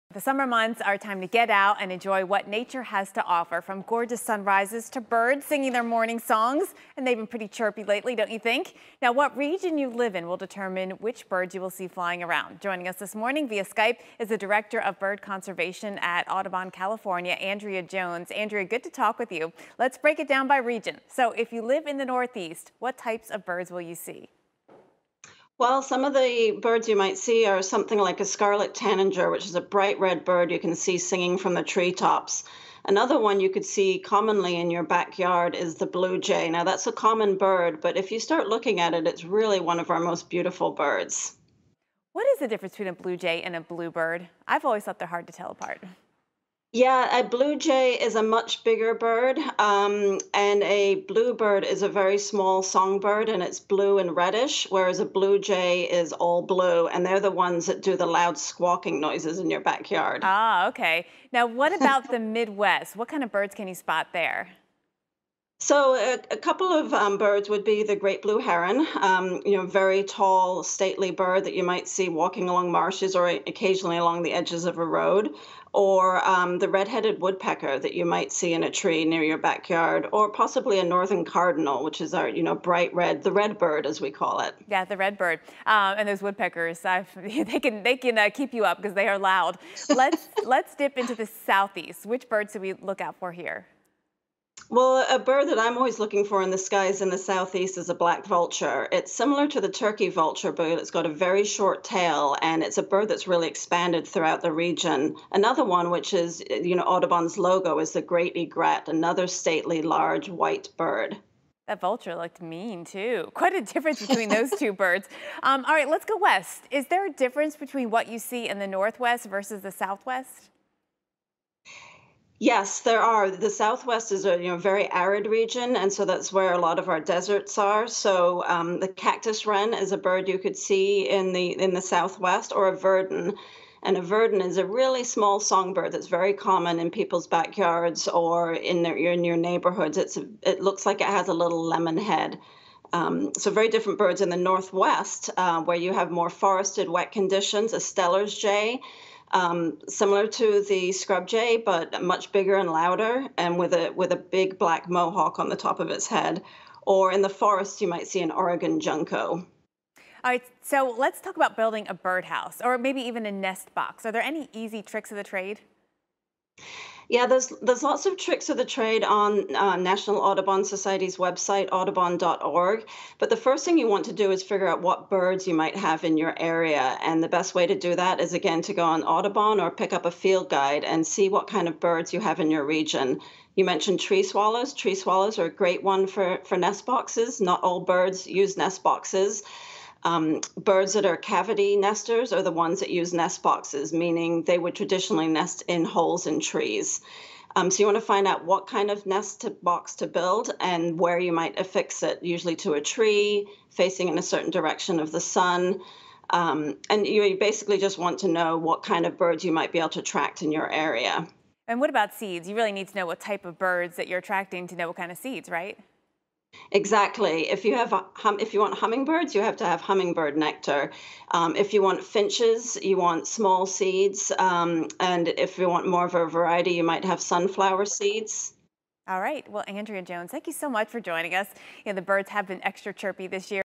Bird Watching Cross Country
Jen Carfagno talks to Director of Birds at Audubon California about where the birds are flying by the region. She also discusses tips and tricks for bird houses, bird boxes and bird feeders.